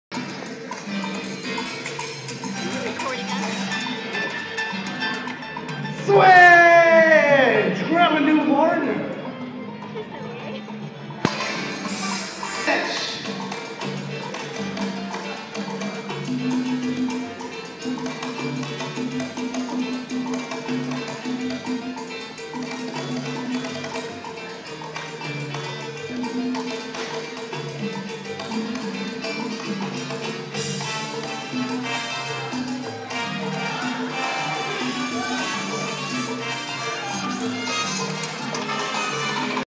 FREE Salsa Dancing Lessons - PSU Lessons November 18th 2010
This routine includes an interesting lead given from the elbow to lead the lady stepping back. Katie is starting healing and we're stepping it up.